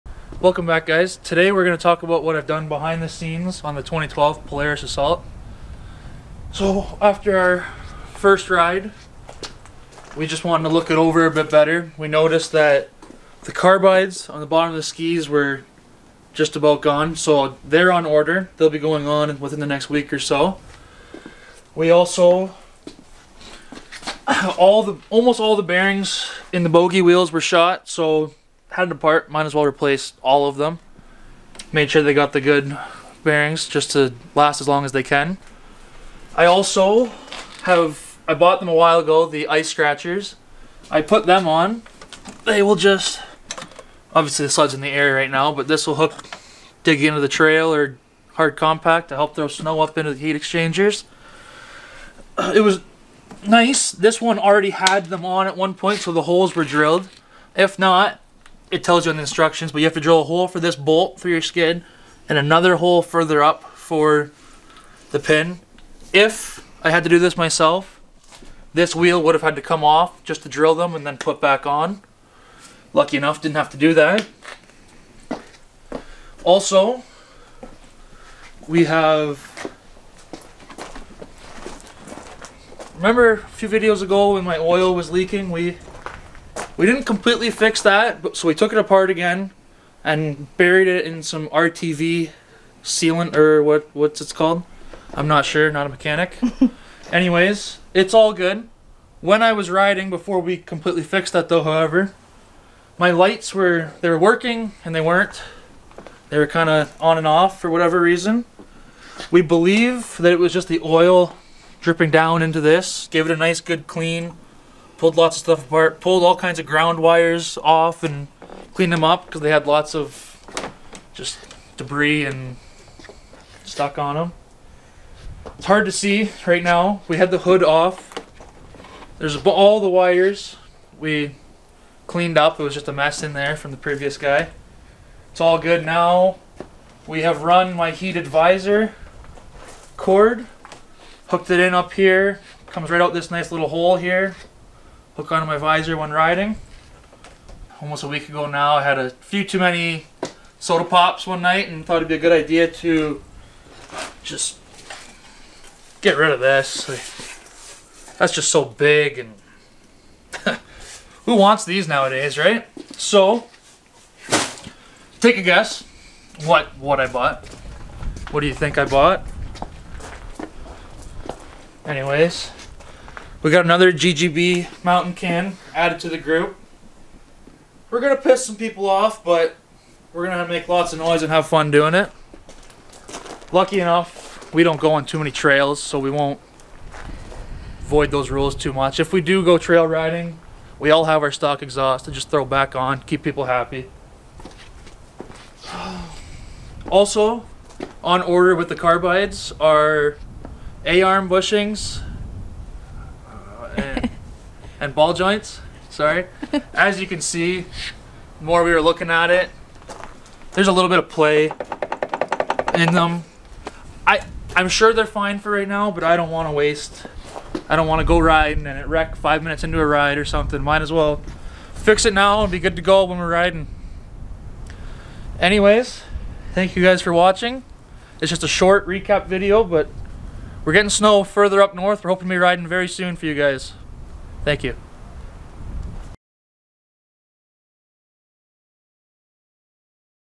Polaris Repair update!
We have fixed this 2012 Polaris assault off camera. We have just posted a quick update to show case all the problems we found. one thing we didn't add in the video is we changed the suspension springs, the ones that were in the skid were not even meant for this snowmobile. Light dimming issue was repaired, bad bearing in the skid, ball joints, bushings, oil leak (again). all these issues were addressed.